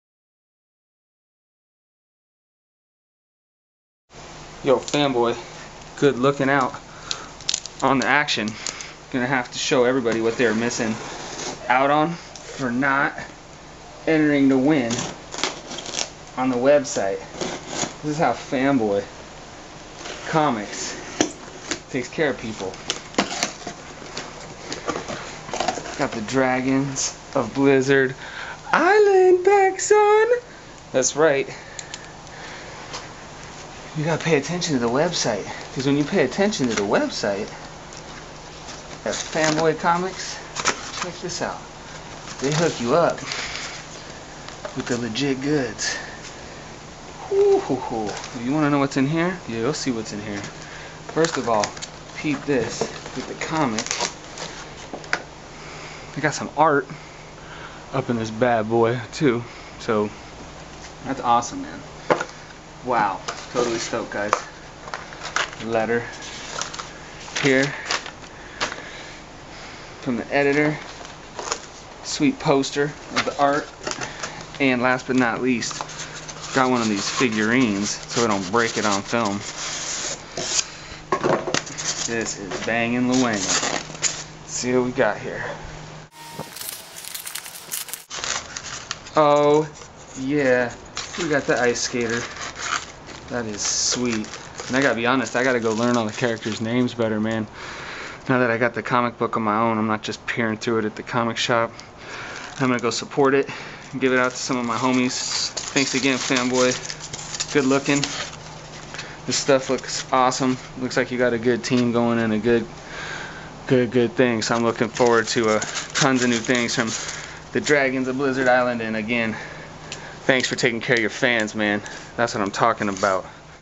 The Dragons Of Blizzard Island FANBOY Comics website contest winner's package
Watch while I open and narrate you on a journey into the contents of a box sent from Fanboy Comics to the winner of a contest the posted on their website to promote their new comic entitled The Dragons Of Blizzard Island.
The Story:
Dragons of Blizzard Island Origins

The plan worked, Eiger stole the chest of Treasure from the Winter Witch and returned it home, to a tiny castle, ruled by an old and once powerful King. The two conspired to return the tiny kingdom back to an empire and rule the lands with civility and humanity. First they had to escape the Witch's wrath.

Eiger conjured a secret island, a safe haven, where they would hide until they regained the power to return. They could not do it without the treasure, but it would take time, something the Witch would not grant them. The Witch's soldiers pursued Eiger back to the castle and laid siege awaiting the full force of their army.

The tiny kingdom could not repel the Witch's army. All they could do is buy time for their King, Eiger and it's treasure to escape to Blizzard Island... want to know more? Go to the web site...see a preview of the book. GREAT art by Matt Brundage...great team put together on this book. Excited to see more from them in the future. Dragons are so cool!